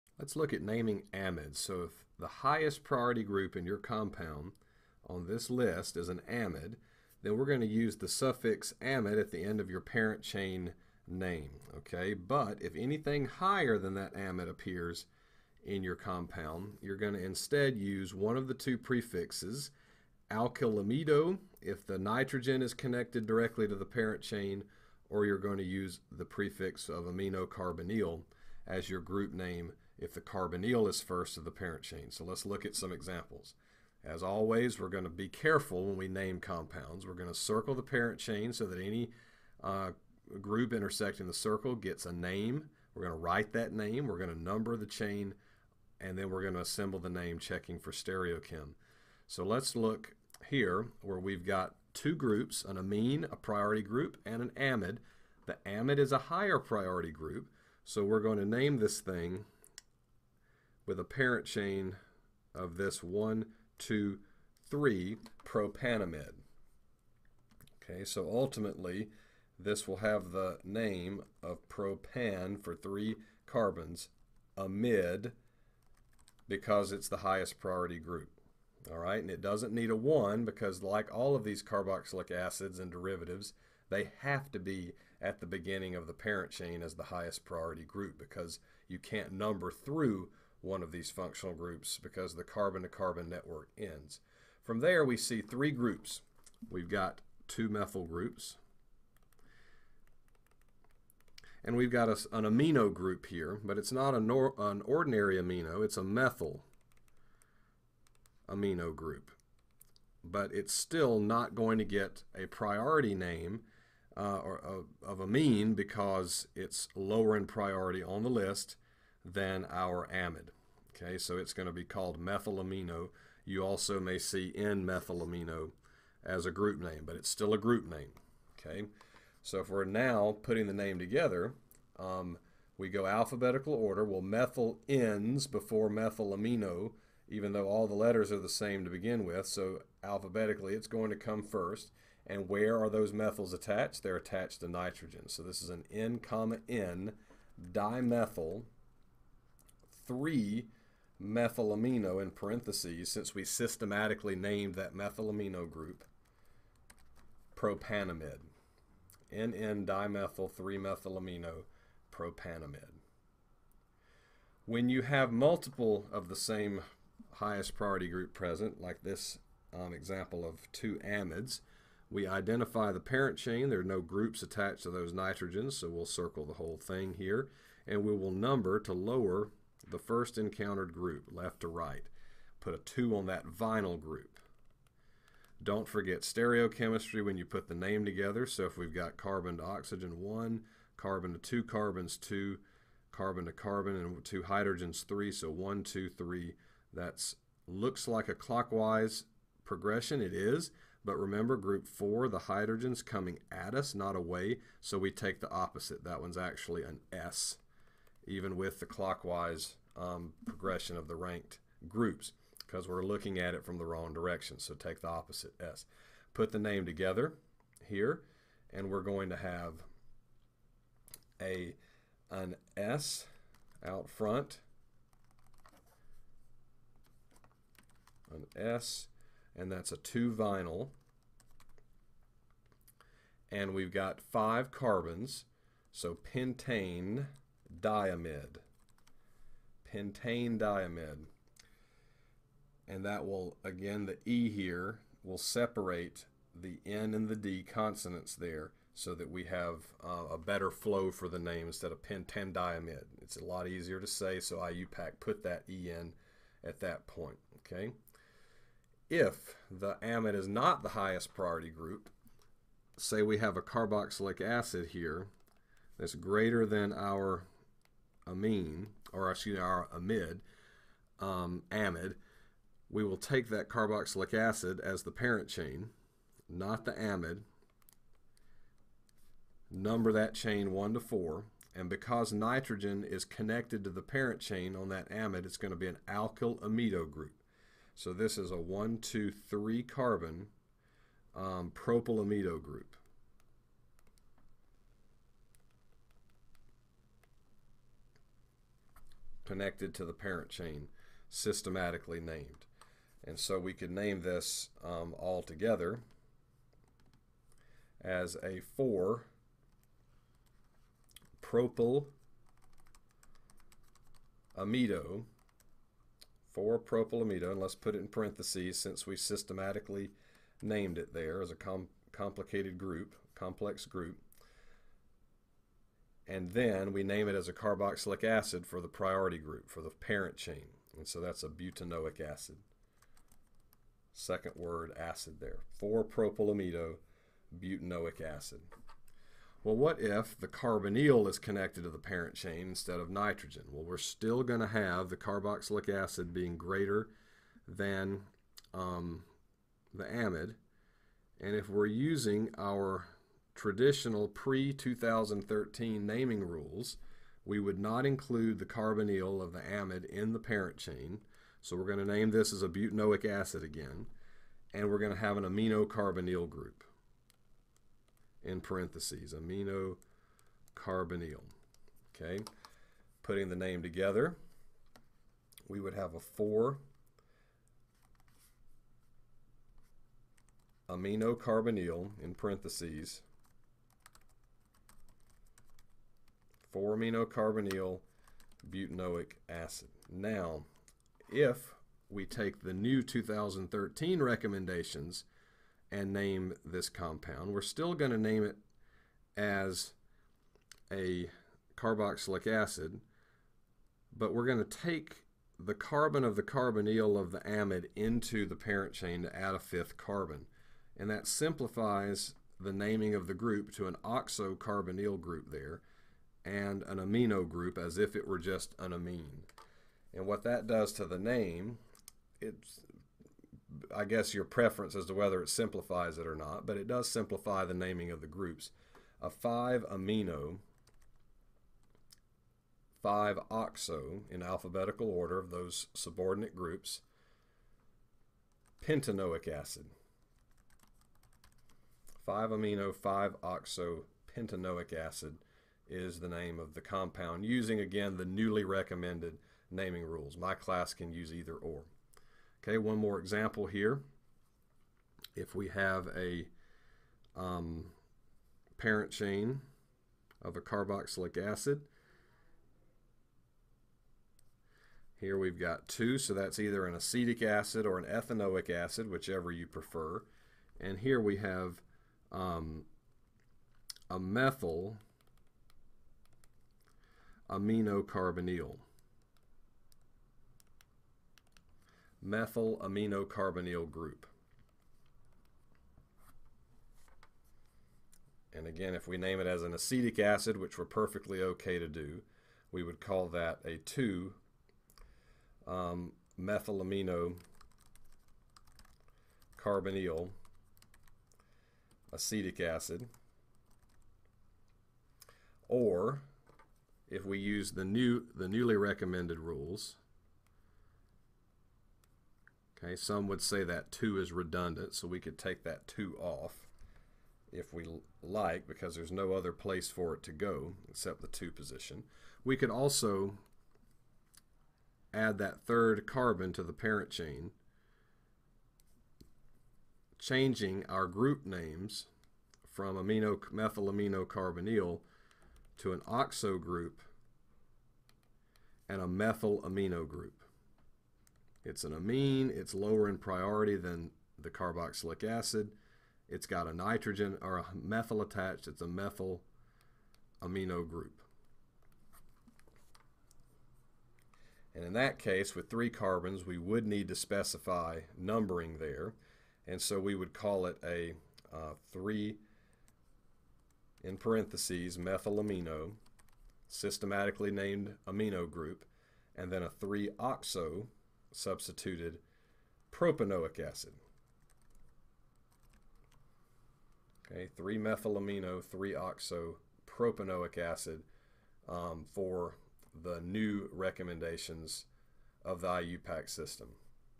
Naming Amides IUPAC Systematic Nomenclature
Looks at naming amides as a priority group and as a subordinate group.  Addresses changes in naming convention introduced in 2013 for groups names.